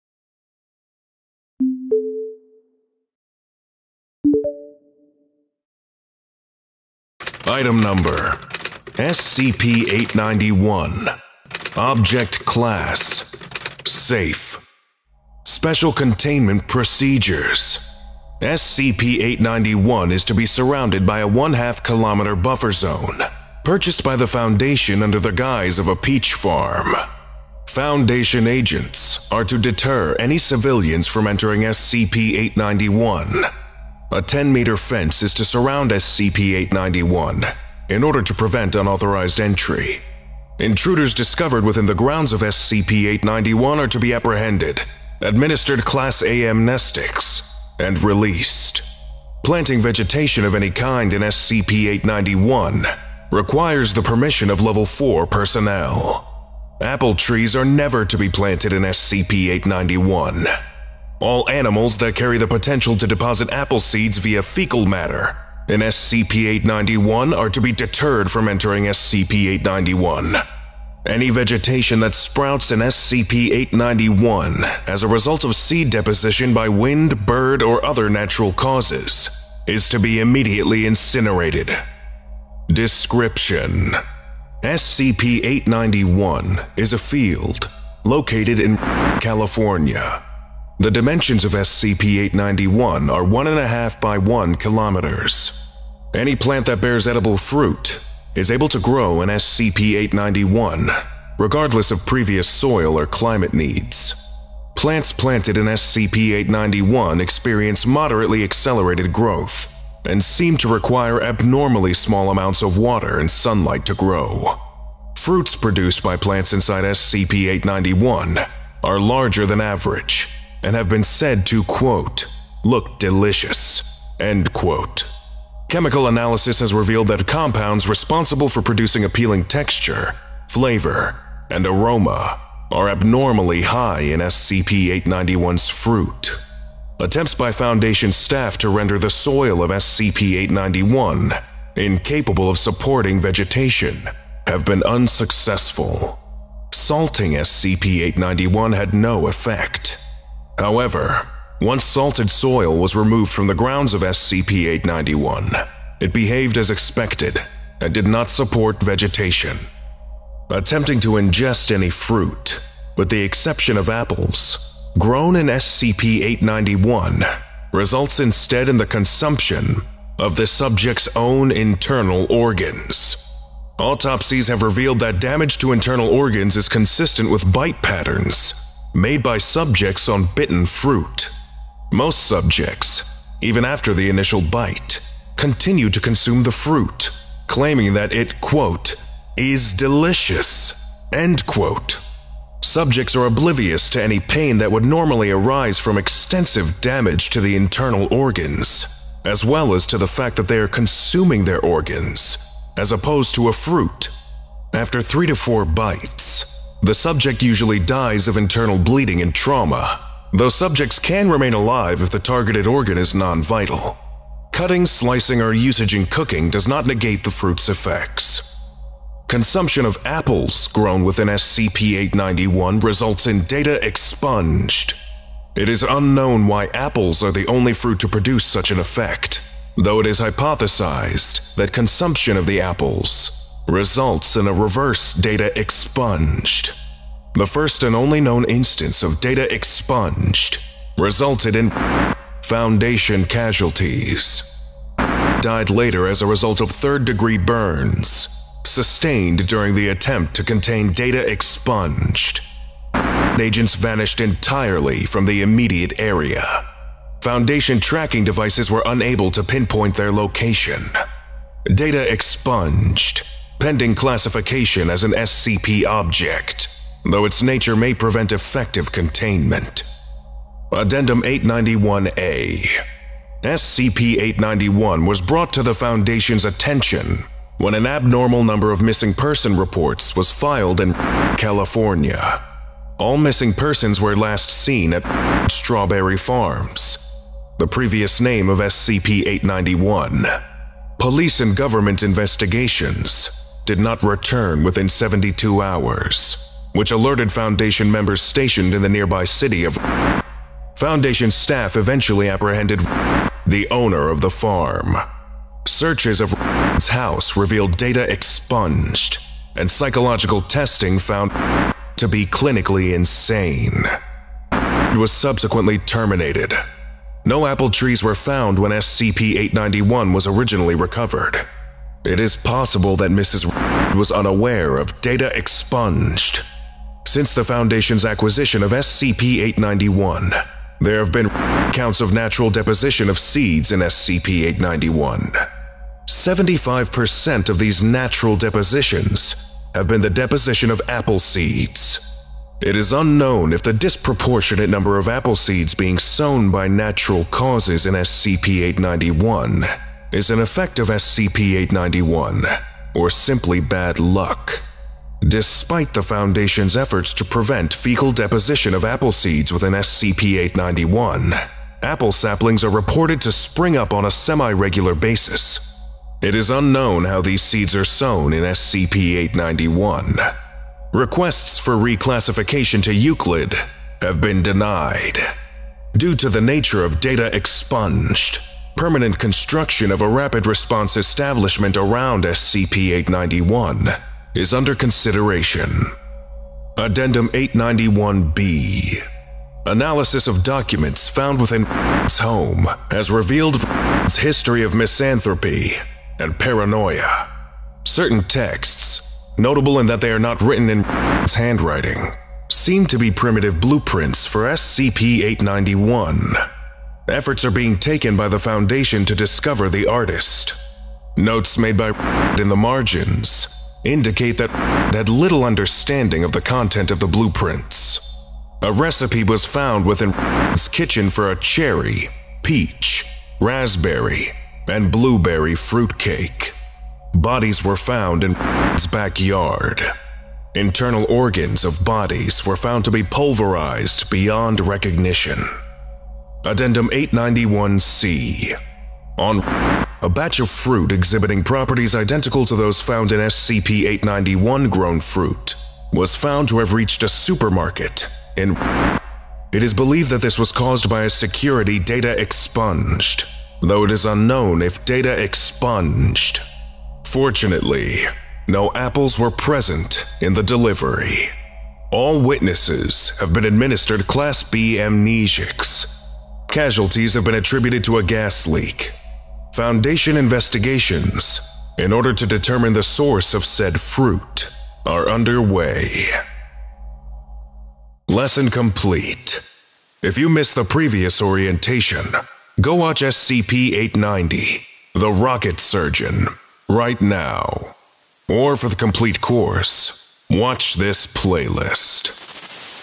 SCP-891 | California Field (SCP Orientation)
Today we will be studying Item number SCP-891: California Field, Object class: Euclid.

SCP-891 is a field located in [REDACTED], California. Any plant that bears edible fruit is able to grow in SCP-891, regardless of previous soil or climate needs. Plants planted in SCP-891 experience moderately accelerated growth and seem to require abnormally small amounts of water and sunlight to grow. Fruits produced by plants inside SCP-891 are larger than average and have been said to look delicious.

Attempting to ingest any fruit, with the exception of apples, grown in SCP-891 results instead in the consumption of the subject's own internal organs. Autopsies have revealed that damage to internal organs is consistent with bite patterns made by subjects on bitten fruit.

Most subjects, even after the initial bite, continue to consume the fruit, claiming that it "is delicious."

and released under Creative Commons Sharealike 3.0. Contributor: Chubert

Author: Lee H, Park J, Seo I, Park S, Kim S